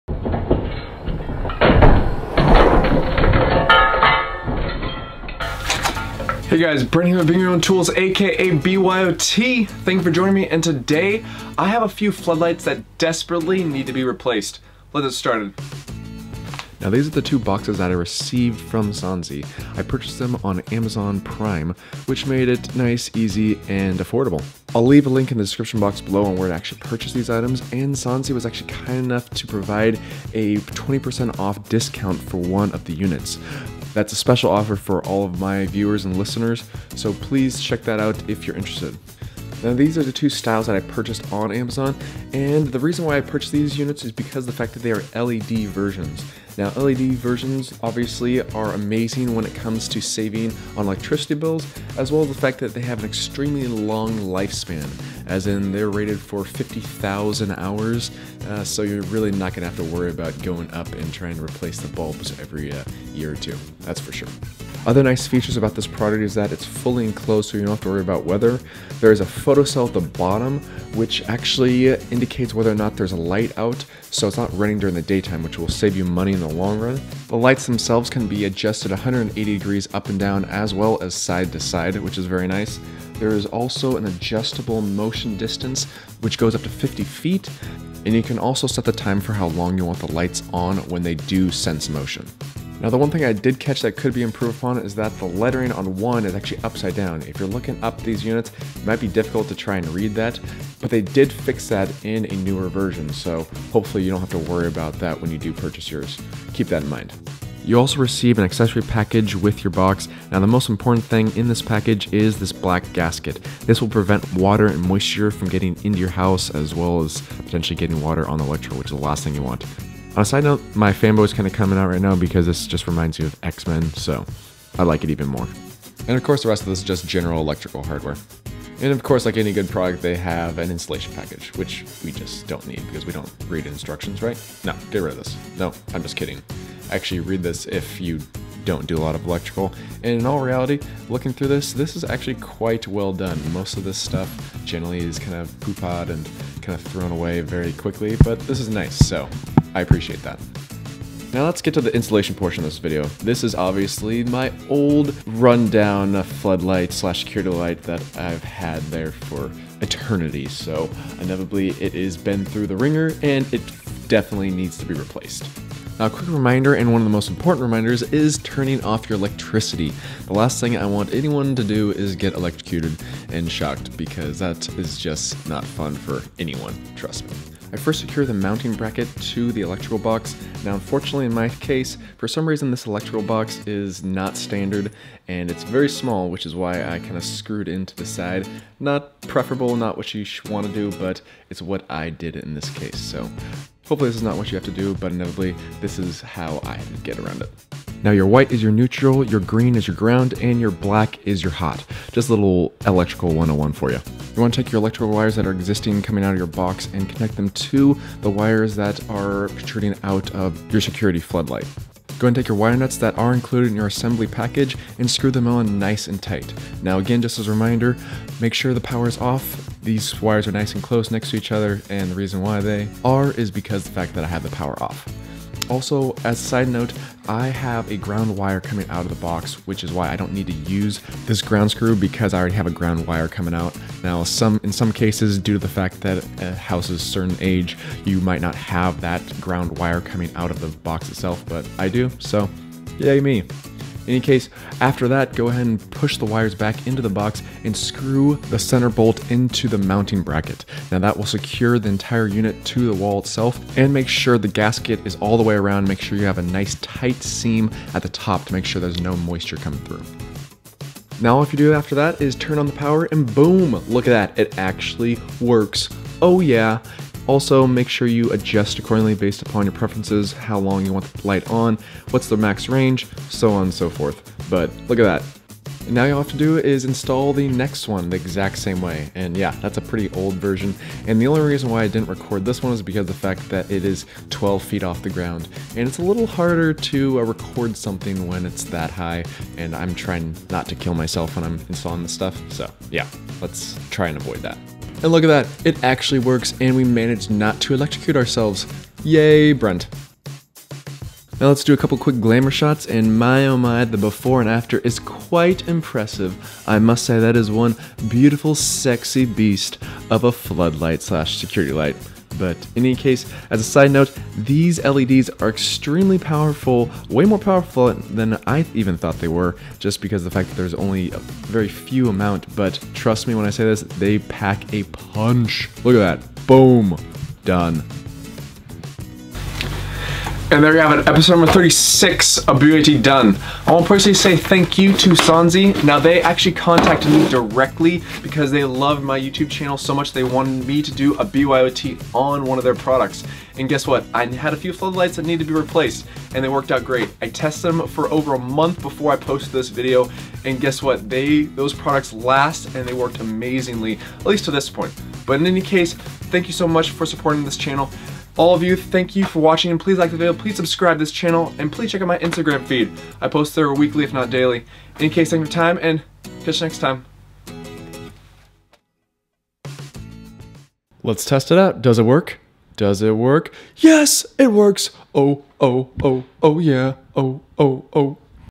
HOW TO INSTALL LED MOTION SENSOR LIGHT : DIY LED FLOOD LIGHT TUTORIAL
This BYOT DIY project is all about how to install a LED motion sensor light or security light.  This is a step by step tutorial on how to install a Sansi LED flood light from start to finish.  These LED flood lights are very easy to install and will save you money and illuminate any area you see fit.  Bring Your Own Tools (BYOTools #36).

Do you link the videos and want to treat Brent to a cup of coffee?

Material List:
-4 LED Light System
-8 LED Light System

20% Savings on 8 LED light System:  youandus